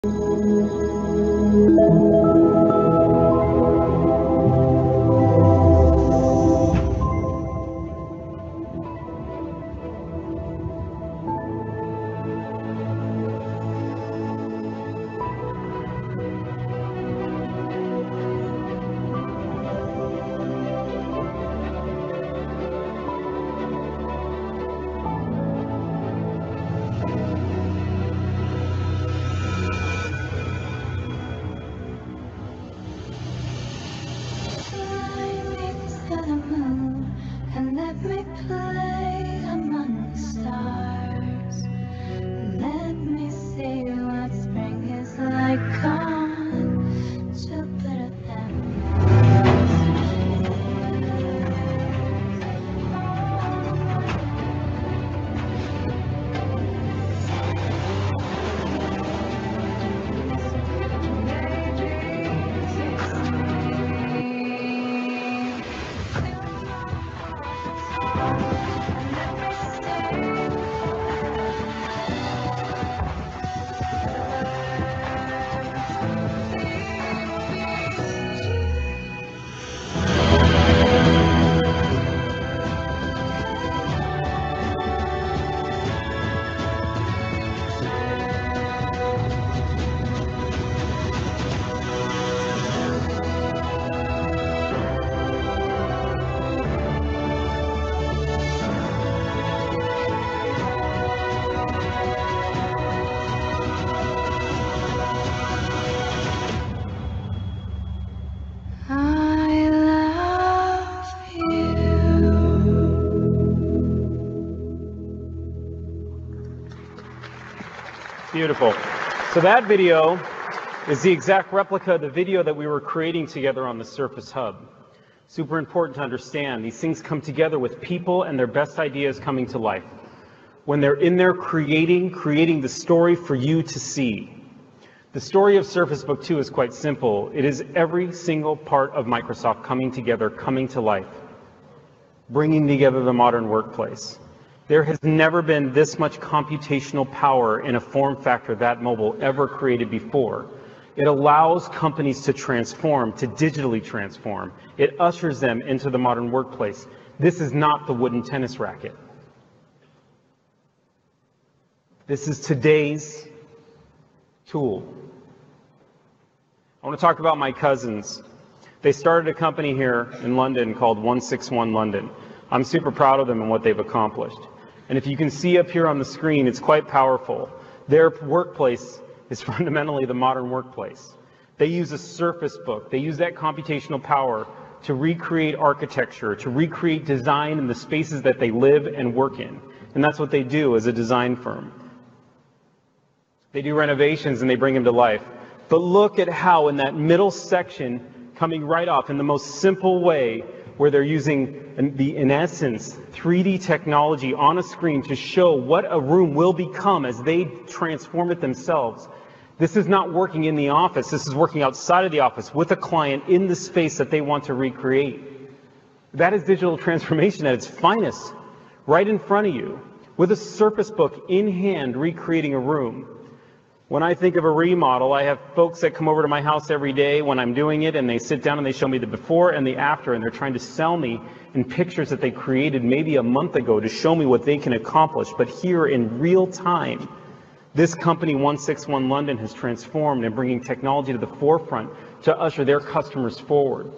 Microsoft Surface Release - Panos Panay Showcasing 1.61 London at the Forefront of Home Technology
Panos Panay releasing Surface Book 2 at his keynote in London. Understanding the modern workplace and how surface is helping companies such as 1.61 London to be at the forefront of technology. Allowing designers to showcase their work in real time 3D outside of the office. 1.61 London uses this technology to showcase their interior design work live on site so clients can better understand the transformation that will take place.